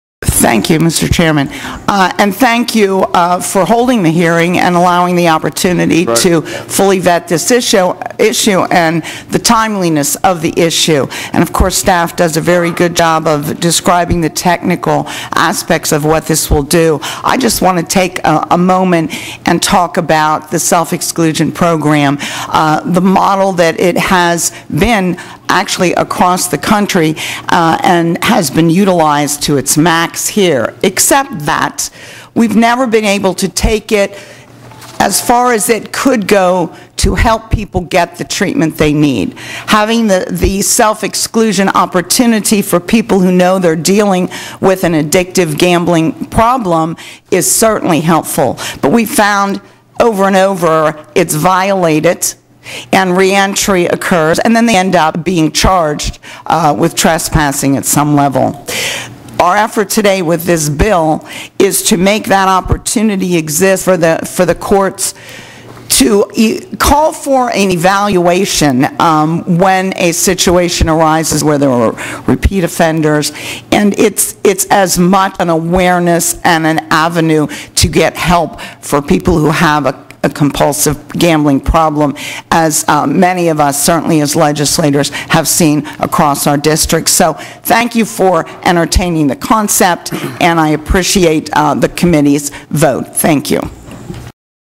Advocating For Self-Exclusion
PA State Rep Mauree Gingrich speaks of House Bill 322 which promotes a process called "self-exclusion."  The bill allows a person to request to be banned from all legalized gaming activities and to be prohibited from collecting any winnings, recovering any losses or accepting complimentary gifts or services or any other thing of value at any licensed facility.